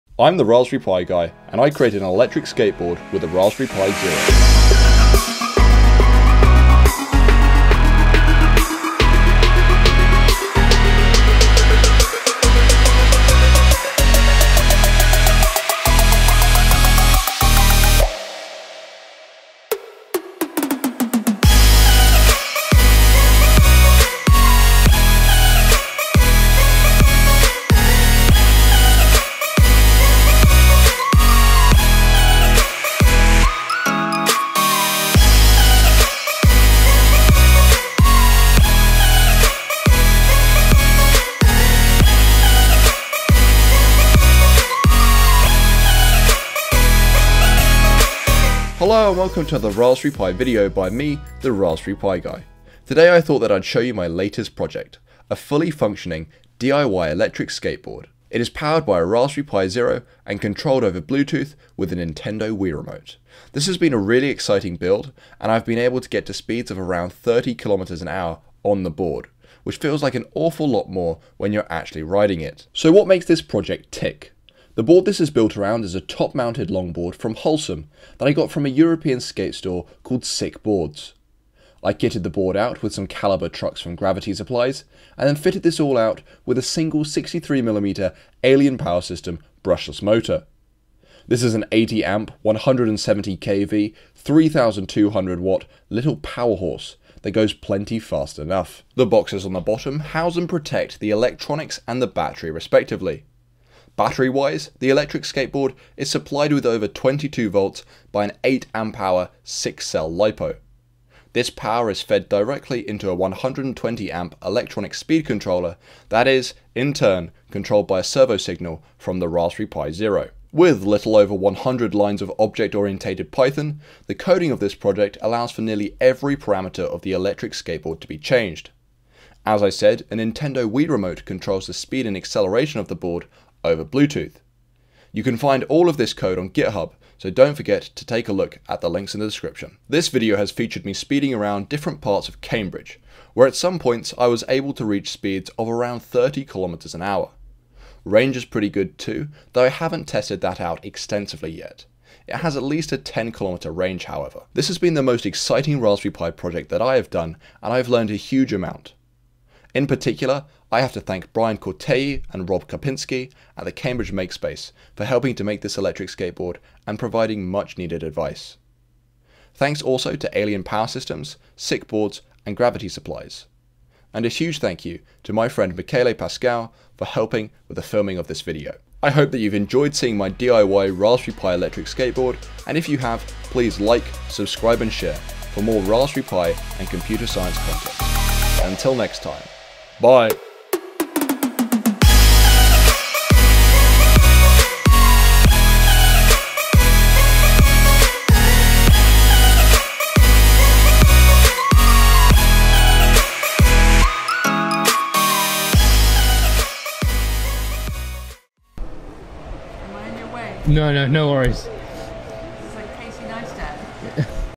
DIY 30KM/H ELECTRIC SKATEBOARD - RASPBERRY PI/WIIMOTE POWERED
Over the summer, I made my own electric skateboard using a £4 Raspberry Pi Zero. Controlled with a Nintendo Wiimote, capable of going 30km/h, and with a range of over 10km, this project has been pretty darn fun. In this video, you see me racing around Cambridge and I explain the ins and outs of this project.

Why make an electric skateboard?! Well... This project was inspired by Casey Neistat, enough said? Also shoutout to one of the original Pi skateboarders, Tim Maier

Huge thank you to Brian Corteil and Rob Karpinski for their help in making this project. Another huge thank you to Michele Pascale for his help in filming this video.

Liked the electric skateboard?! Want to see more cool content?!

The Raspberry Pi Guy

Shot in Cambridge in 4K using the Panasonic Lumix GH3 and G7.